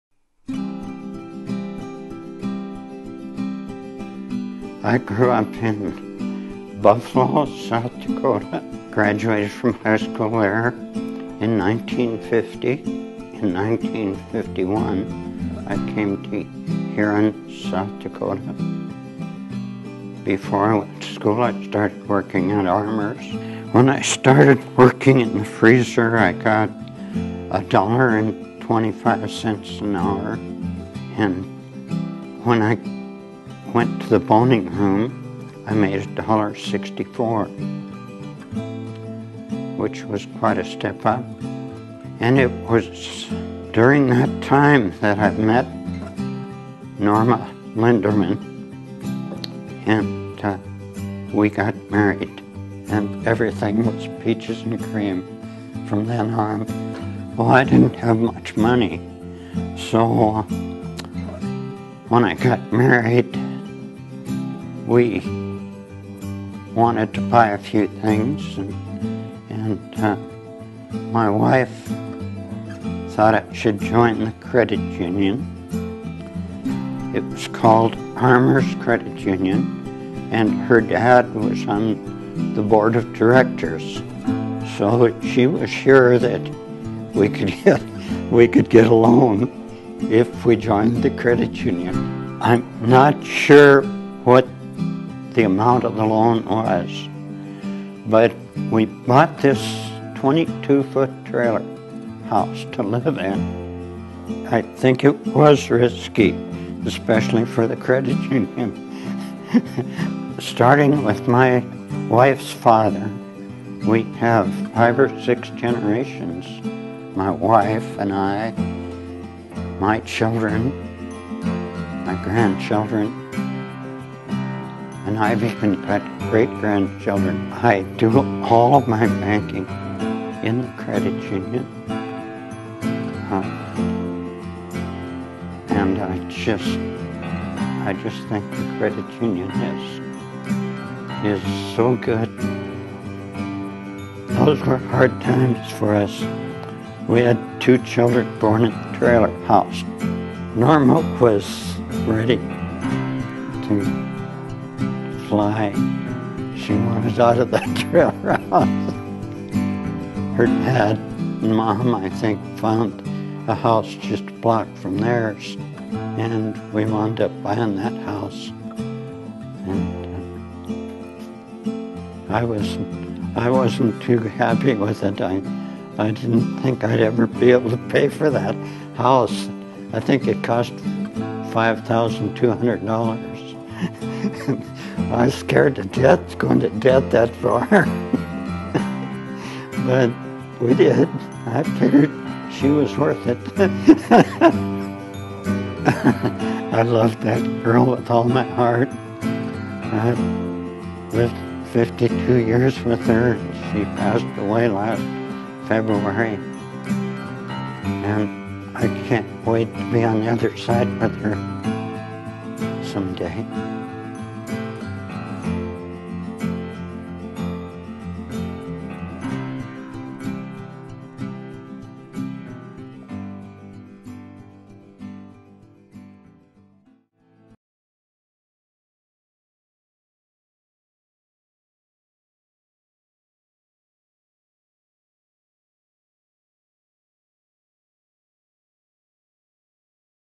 Dakotaland Federal Credit Union's 80th Anniversary
In honor of Dakotaland Federal Credit Union's 80th Anniversary, this video was created to reflect upon our rich history with an inspiring perspective of the influence the credit union has had over one man's life and his family.